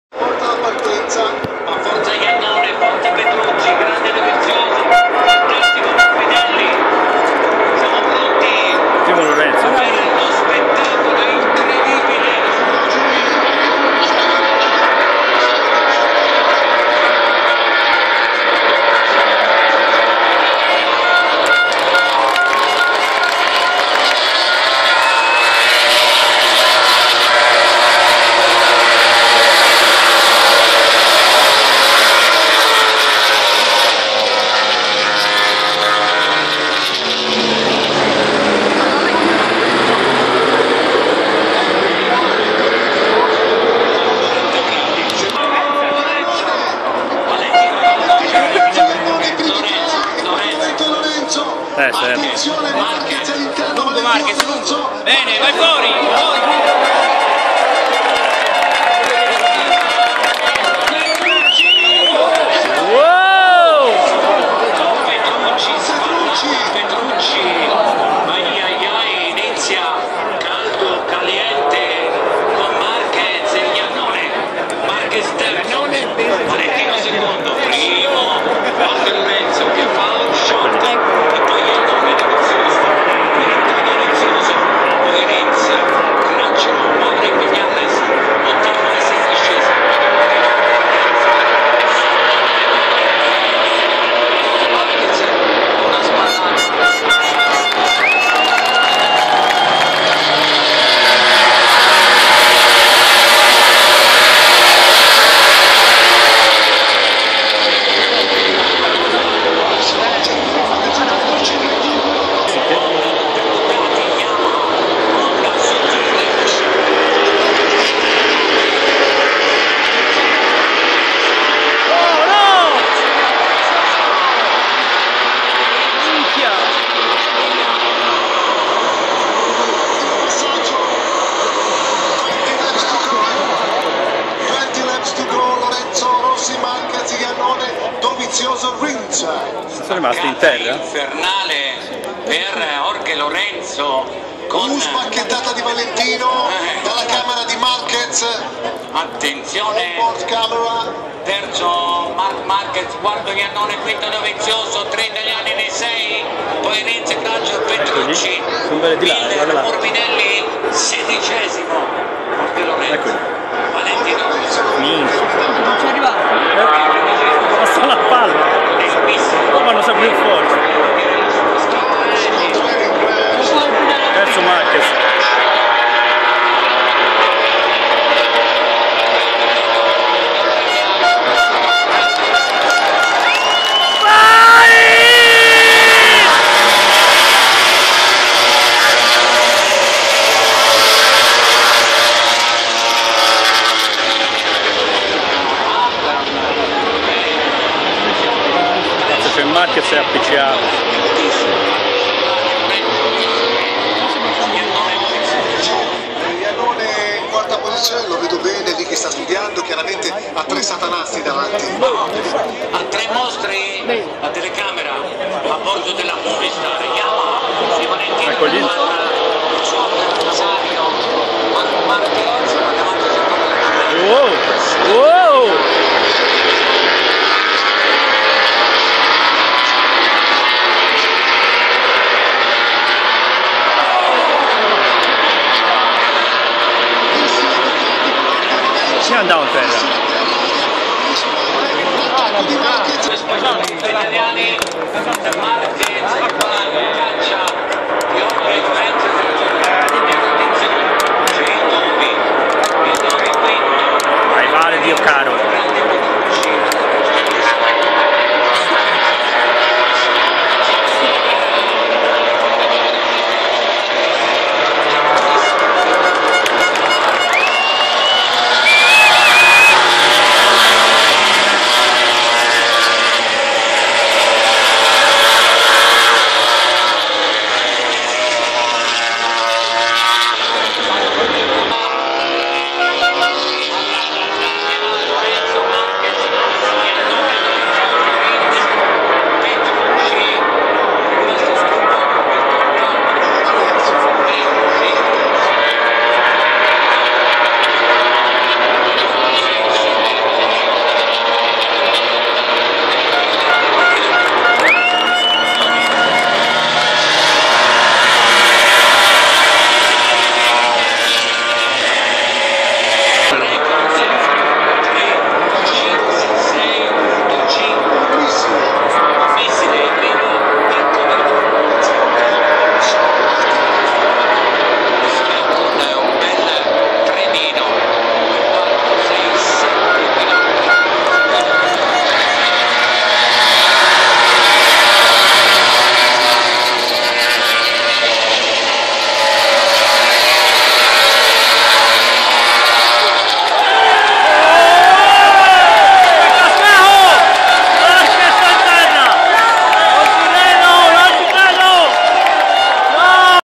Mugello MotoGP 2018
I primi giri del moto GP d' Italia 2018 al Mugello, fino alla caduta di Marc Marquez. Incidenti e cadute non viste in TV